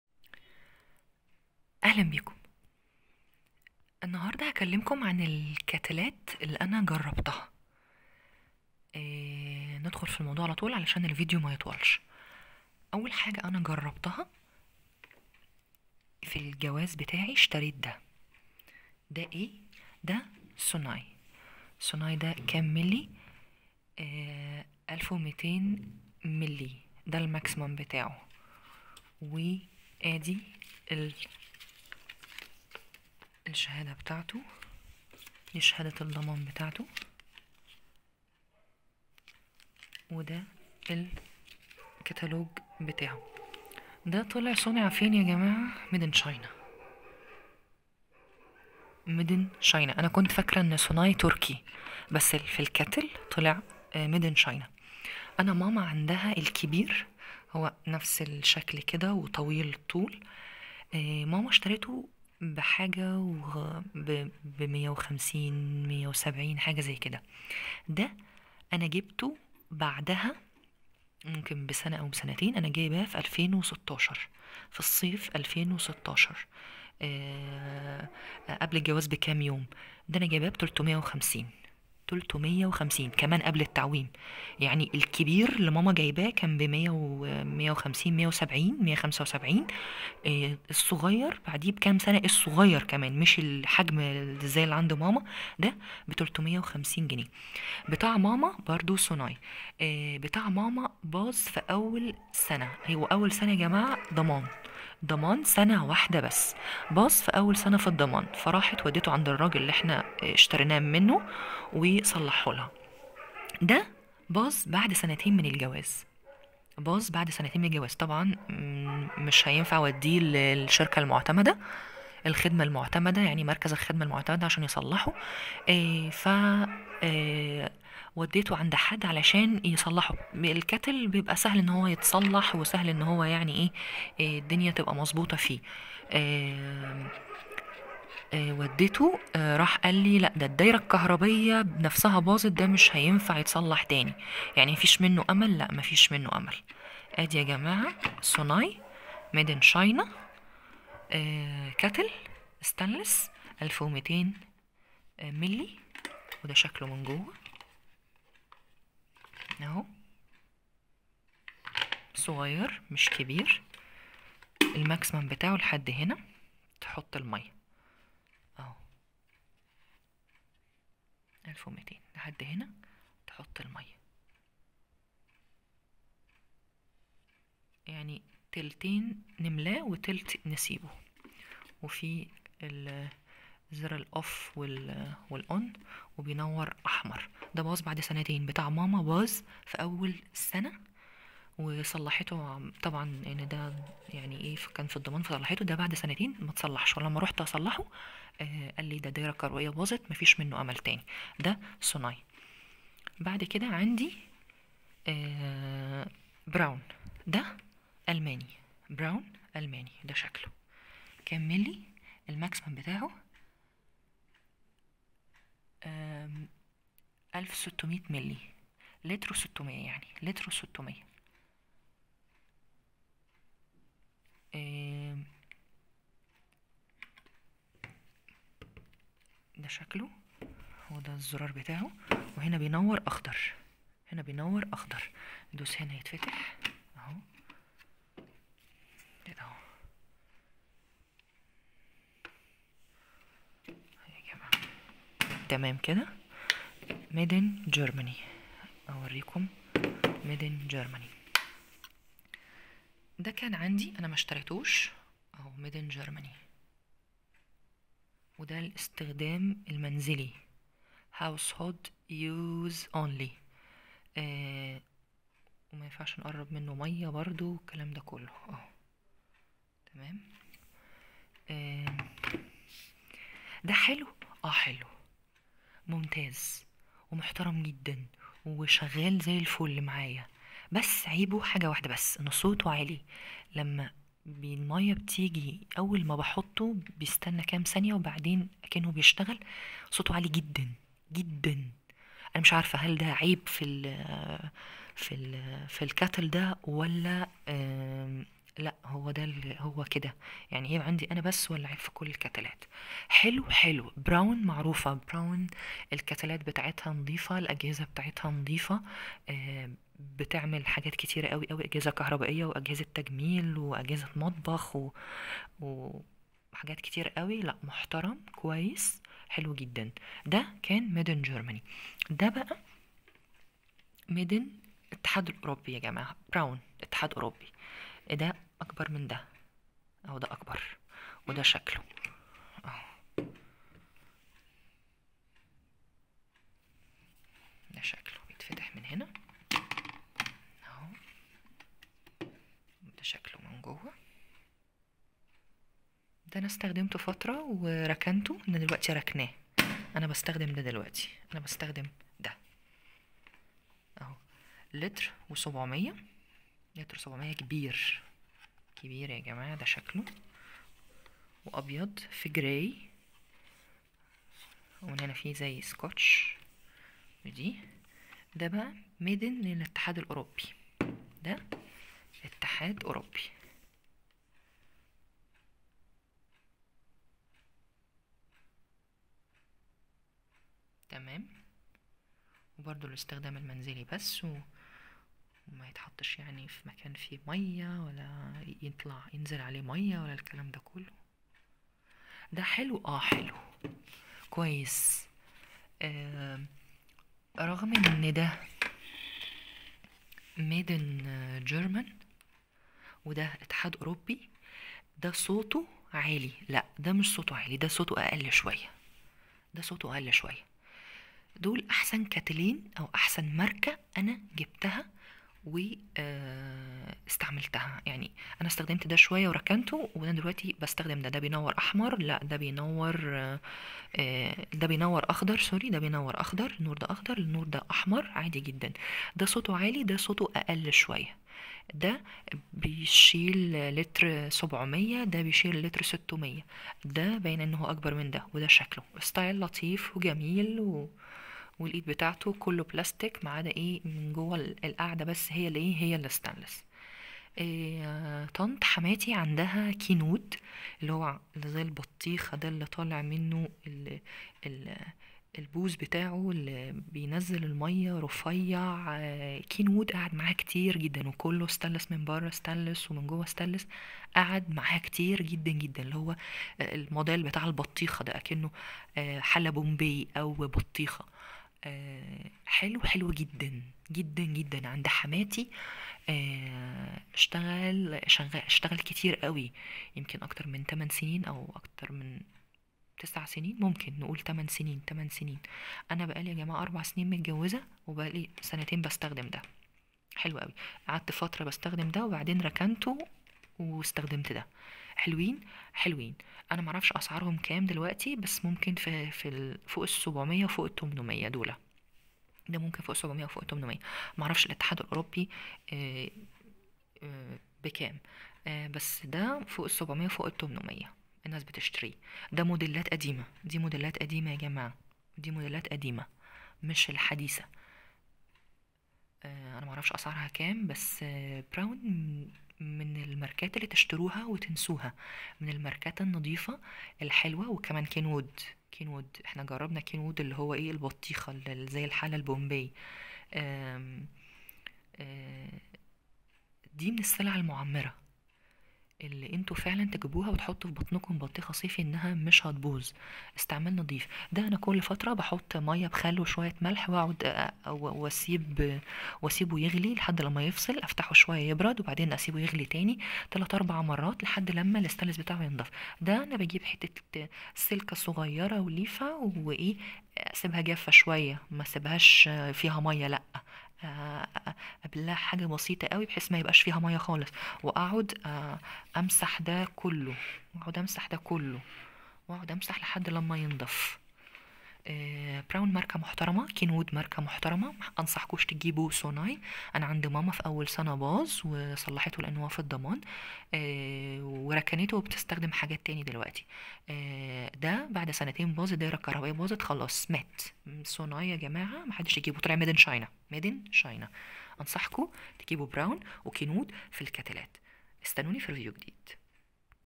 افضل نوع كاتيل غلاية ؟ كاتيل براون المانى كاتيل براون اتحاد أوروبية كاتيل كينوود كاتيل سوناى صينى
الفرق بين غلاية سوناى صينى ضمان سنة والفرق بين غلاية براون المانى والفرق بين غلاية براون اتحاد اوروبى والفرق بين غلاية كينوود موديل البطيخة أو البومبيه...